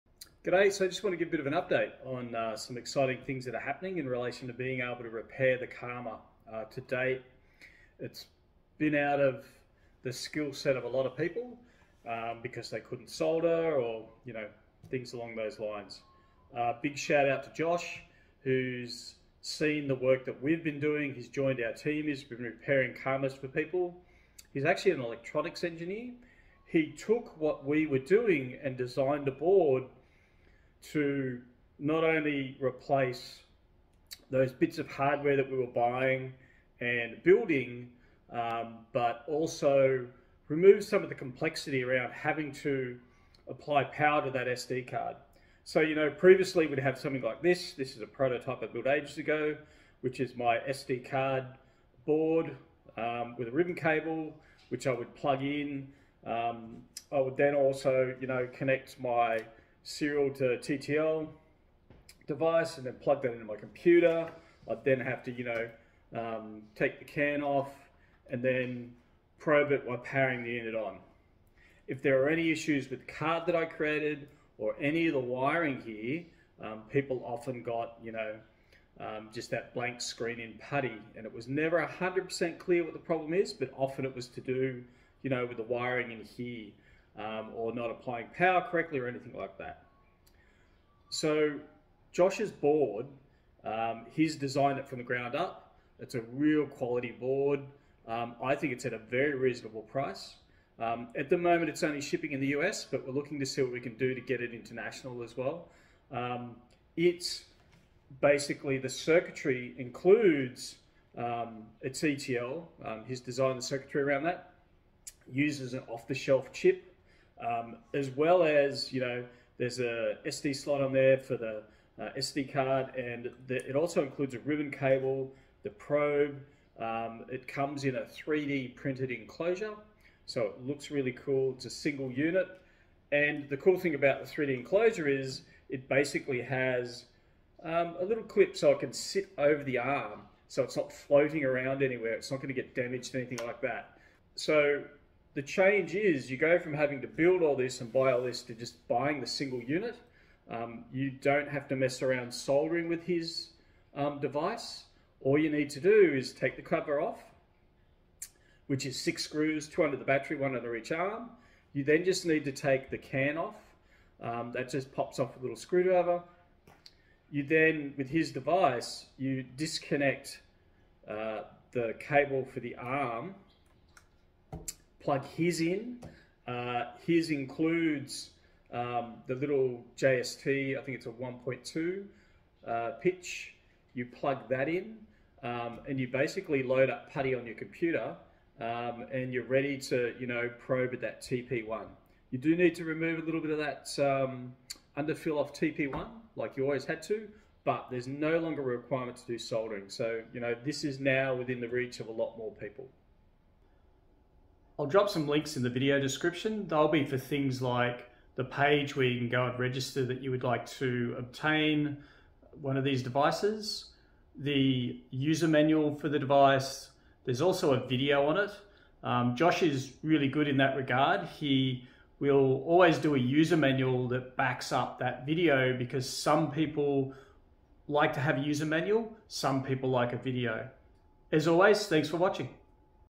Project Good Karma - GoPro karma pairing fix in a box
This device simplifies the pairing fix removing the need for any soldering and provides an easy-to-use program to run the commands.